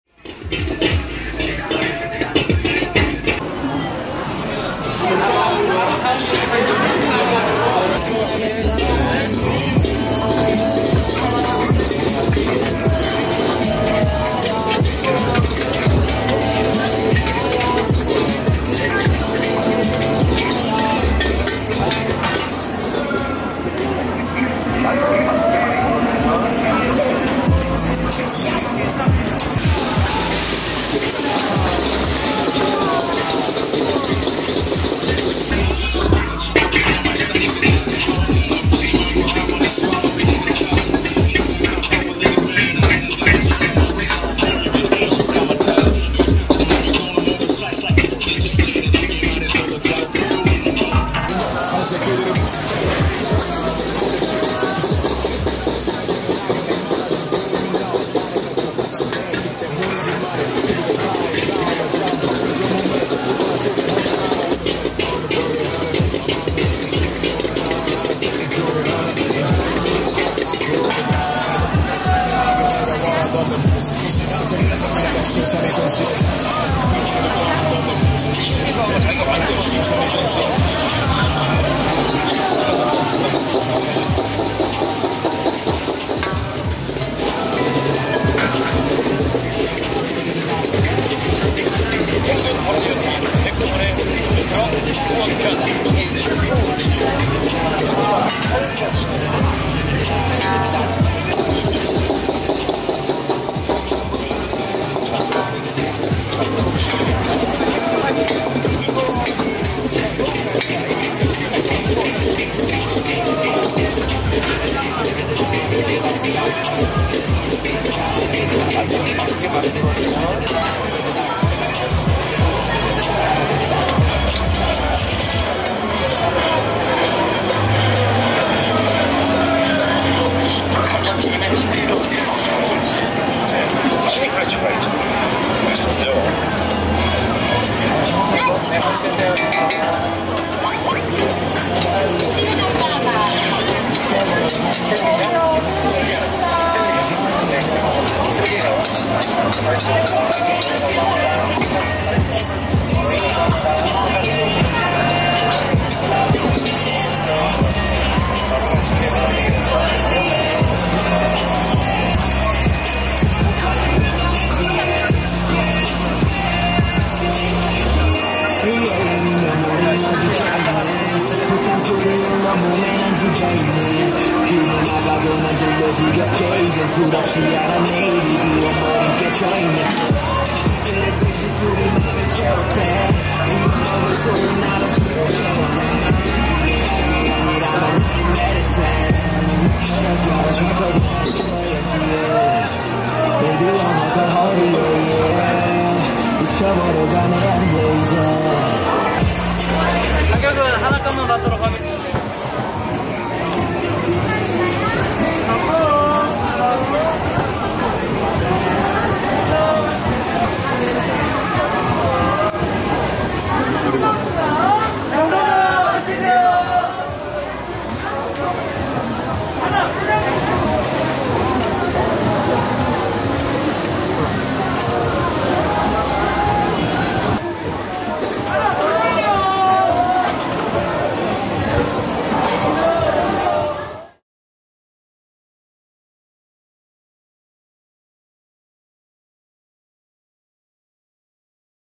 진한 불향 가득 제주 흑돼지볶음면 Jeju black pork Stir-fried Noodle / Korean street food
진한 불향 가득 제주 흑돼지볶음면

제주 동문시장 초면
흑돼지볶음면 7,000 won (usd 5.92)